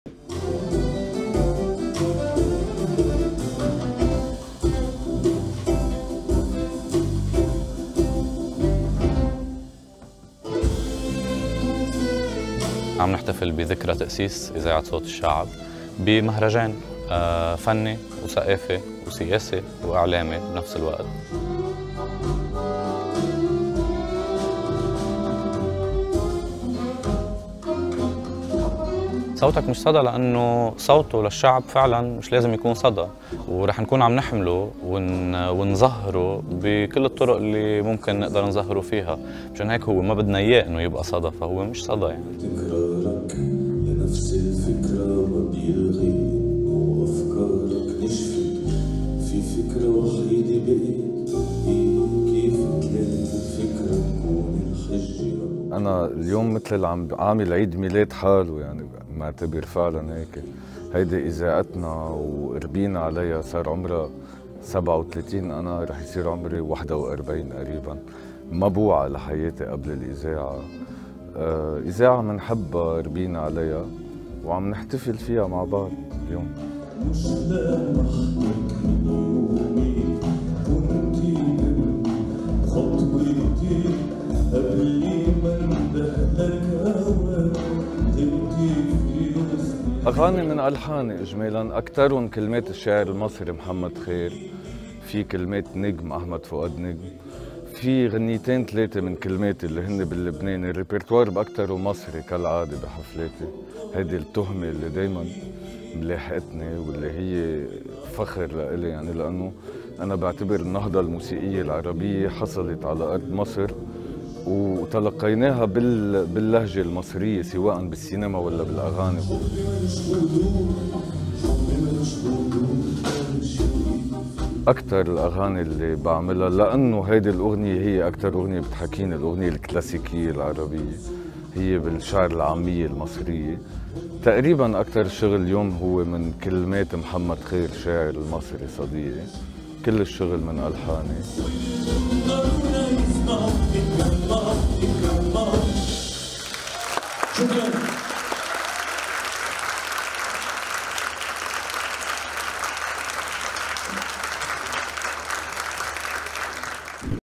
زياد سحاب يحيي الذكرى السادسة والثلاثين لإنطلاقة إذاعة صوت الشعب
في بيروت على مسرح المدينة، أقيم مهرجان "صوتك مش صدى" للاحتفال بالذكرى السادسة والثلاثين لتأسيس إذاعة صوت الشعب.

ضمن فعاليات المهرجان قدم الفنان زياد سحاب حفلاً لأشهر أغنياته  إضافة إلى أغنيات قدمها للمرة الأولى..